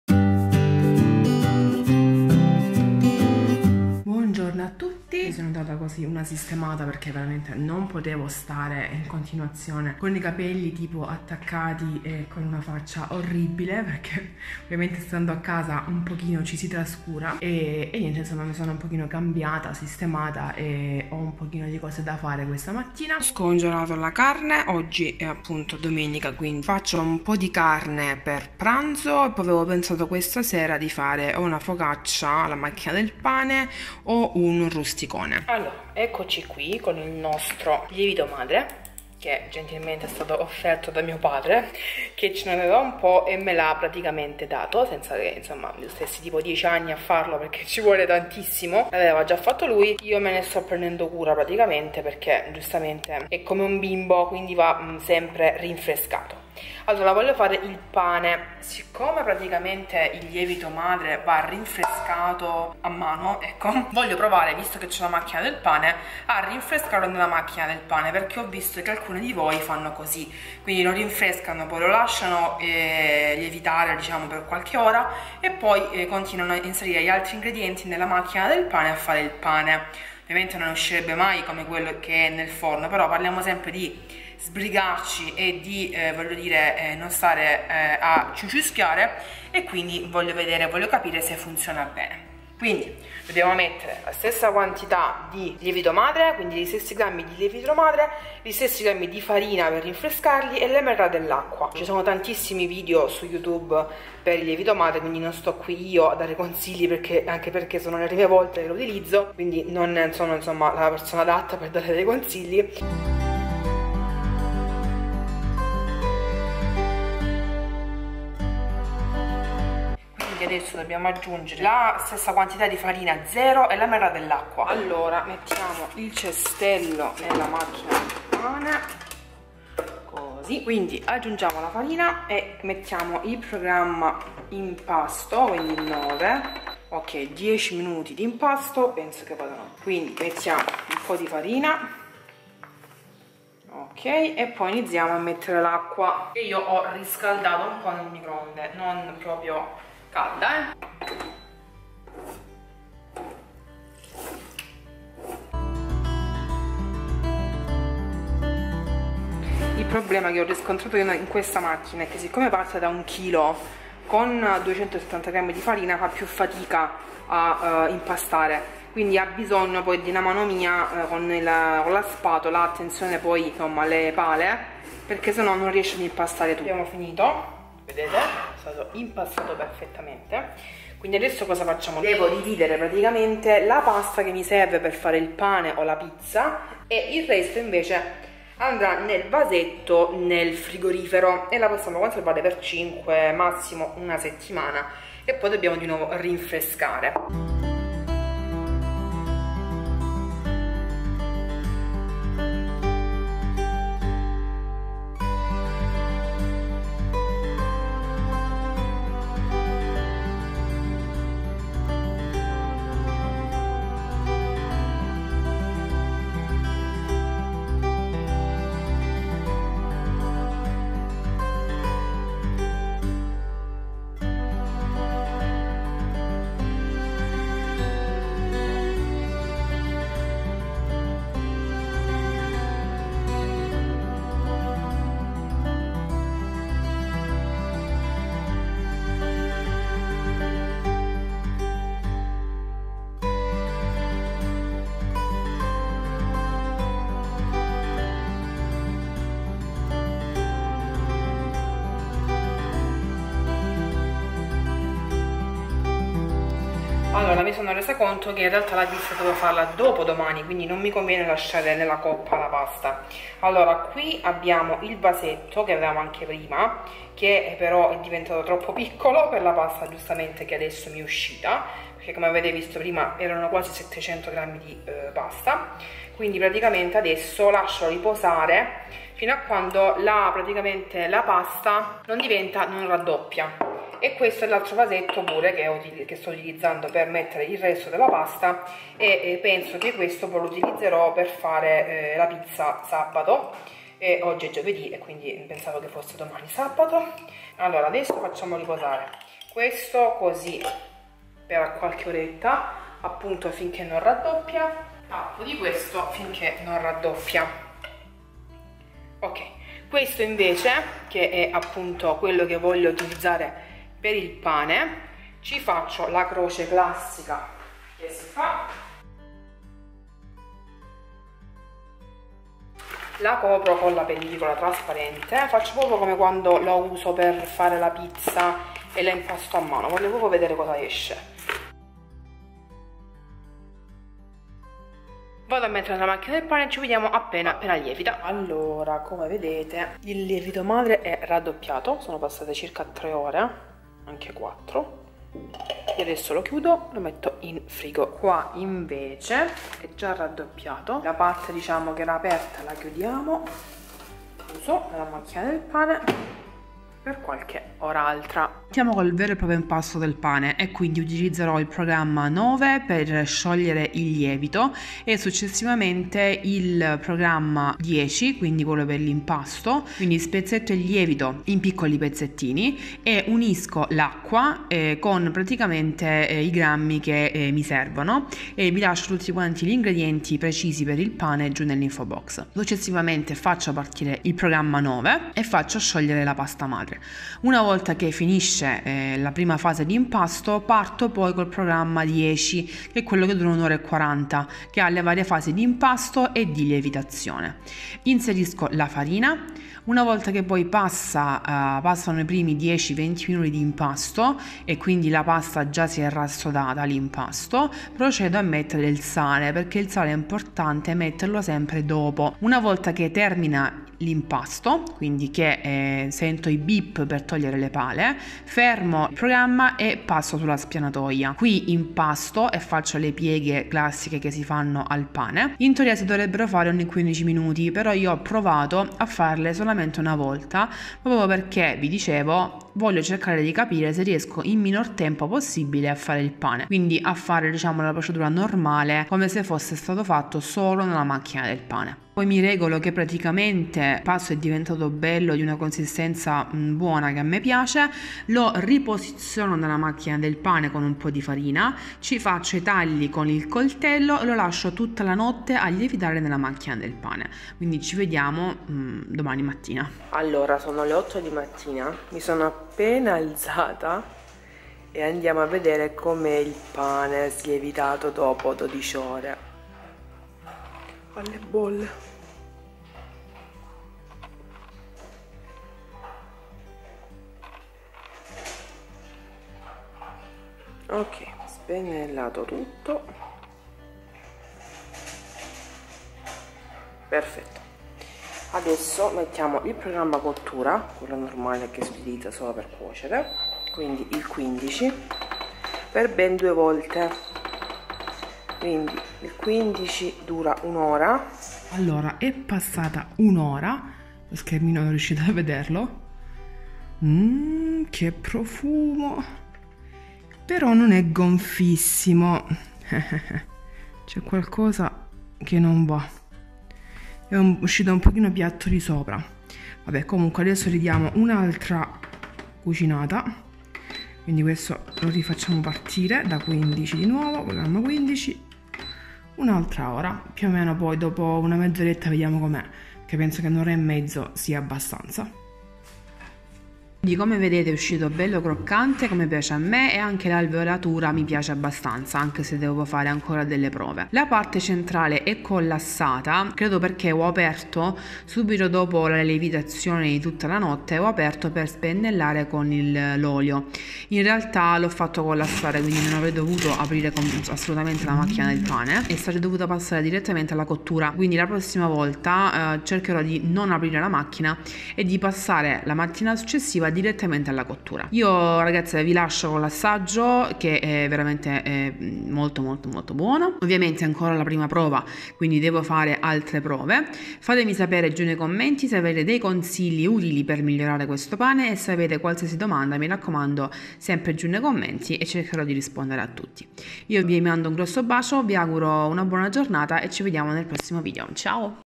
Pane con Lievito Madre nella Macchina del Pane. Proviamo a farlo insieme!
Ciao a tutti! Oggi proviamo a fare il pane con il LIEVITO MADRE nella Macchina del Pane. Cosa verrà fuori?

MUSICA